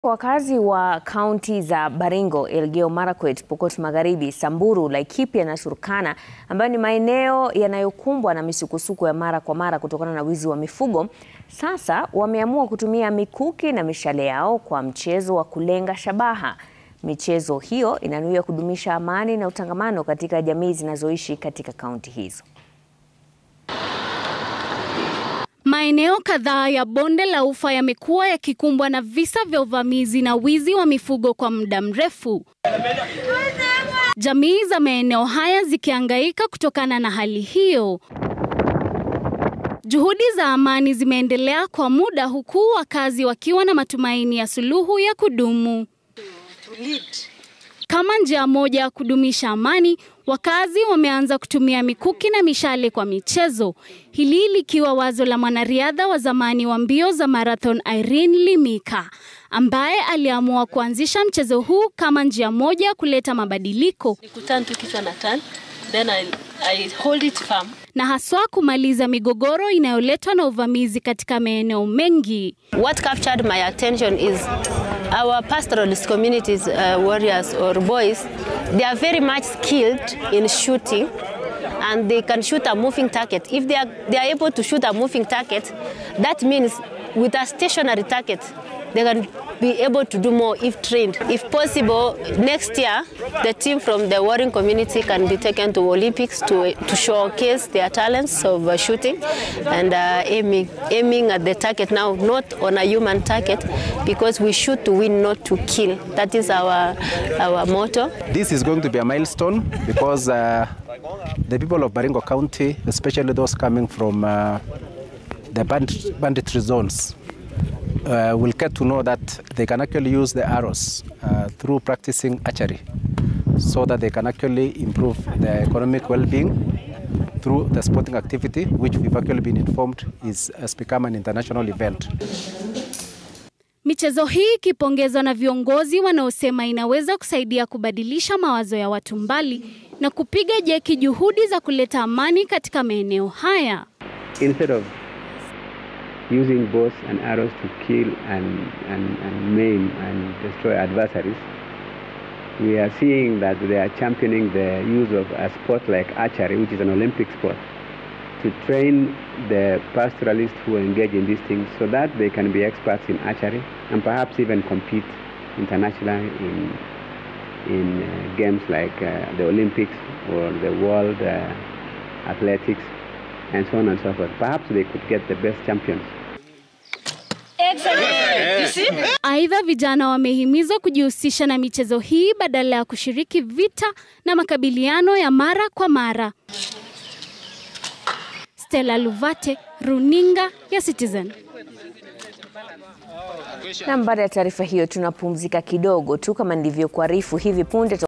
Wakazi wa Baringo wazamia ulengaji shabaha kudumisha amani
Wakazi wa Kaunti za Baringo, Elgeyo marakwet, Pokot Magharibi, samburu,laikipia na Turkana, ambayo ni maeneo yanayokumbwa na misukosuko ya mara kwa mara kutokana na wizi wa mifugo sasa wameamua kutumia mikuki na mishale yao kwamchezo wa kulenga shabaha. michezo hiyo inanuiwa kudumisha amani na utangamano katika jamii zinazoishi katika kaunti hizo.